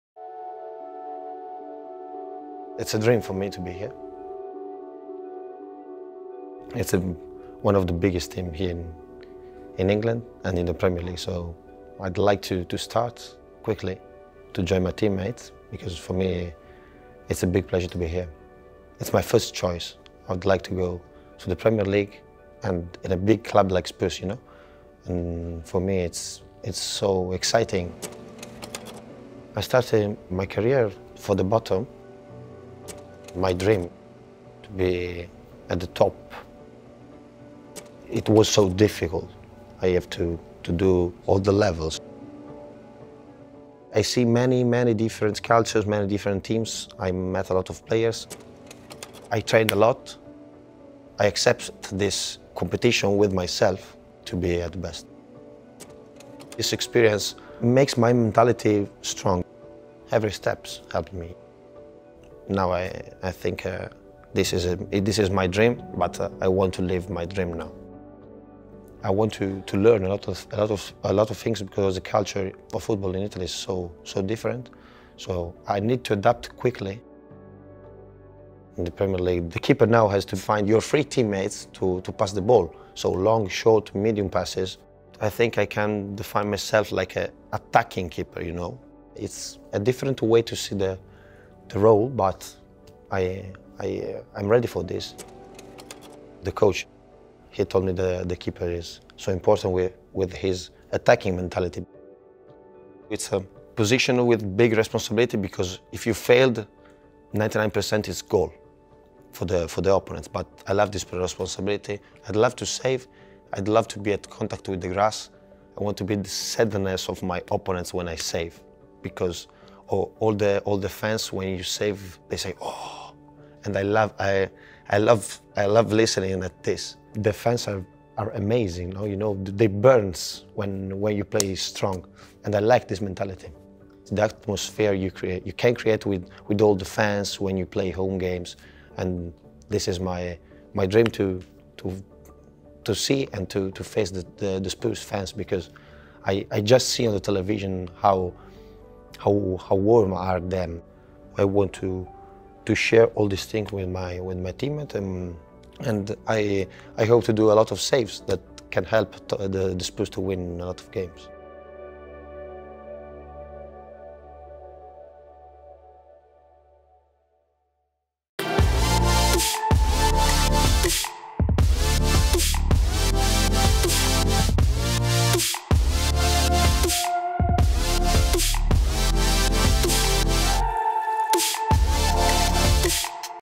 GUGLIELMO VICARIO'S FIRST INTERVIEW AT TOTTENHAM HOTSPUR
Watch as Guglielmo Vicario gives his first interview as a Tottenham Hotspur player after completing a transfer from Empoli.